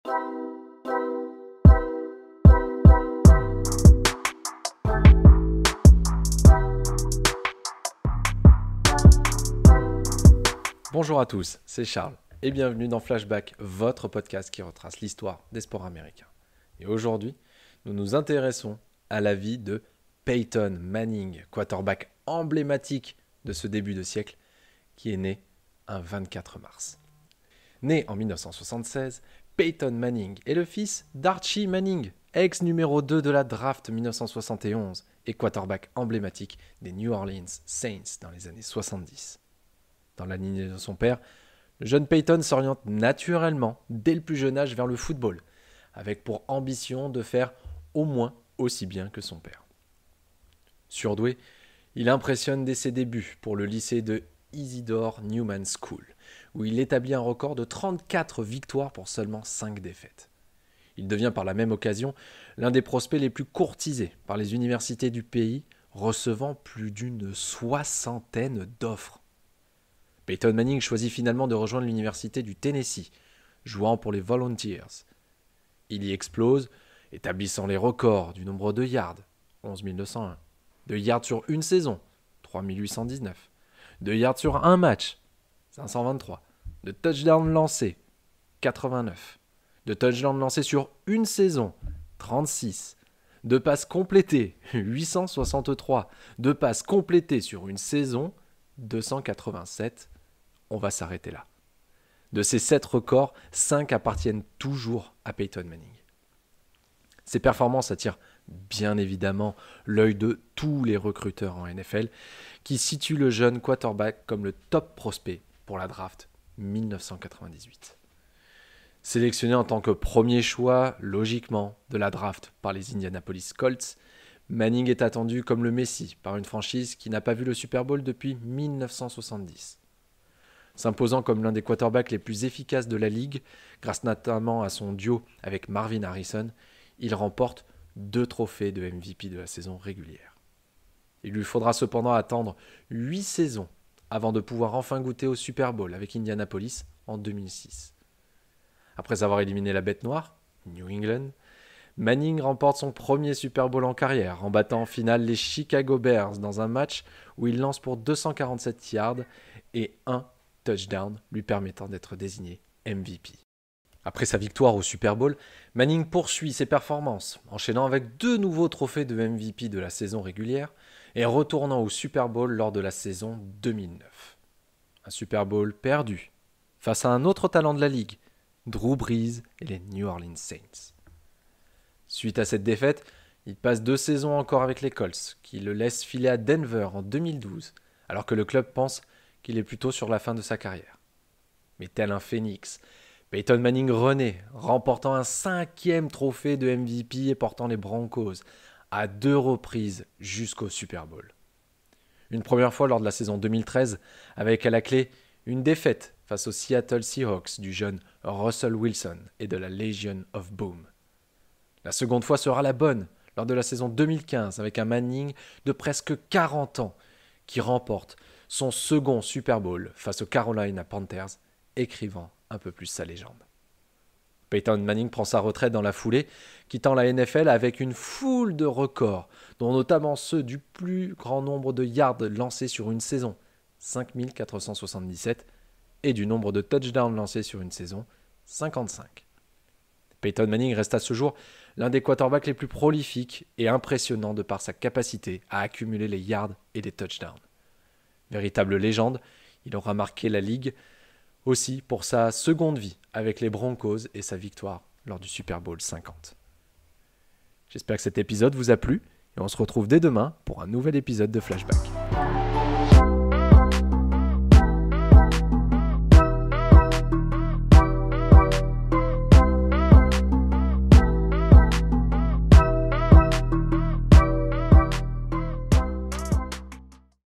Flashback : naissance de Peyton Manning, quarterback mythique
Dans notre série Flashback, nous revenons sur les grandes dates de l’histoire du sport américain et aujourd'hui nous nous intéressons à la vie de Peyton Manning , quarterback emblématique de ce début de siècle naît un 24 mars.